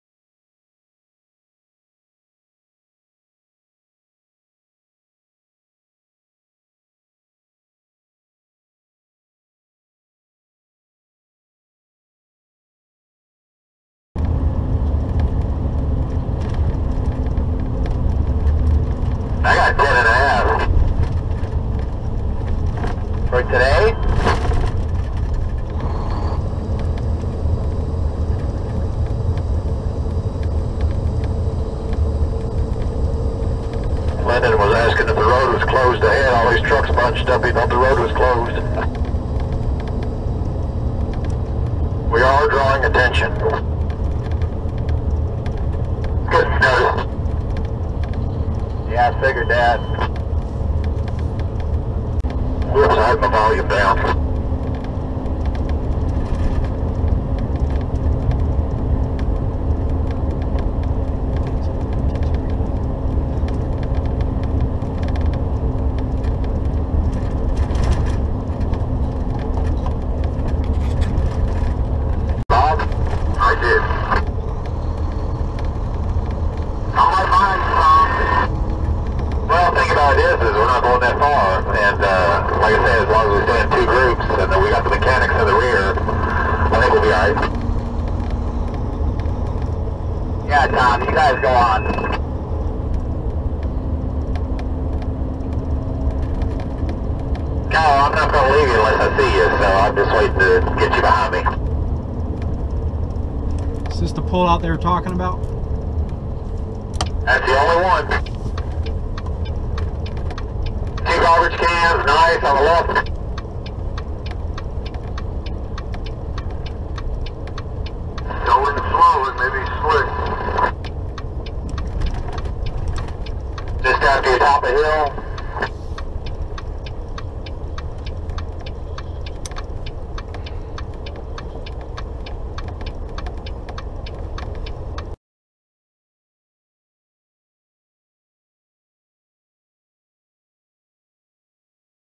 2015 VOLVO 670 LANDSTAR TRIP 54 DAY 5 PART 8 REST AREA
STOPPING AT A REST AREA IN YUKON BC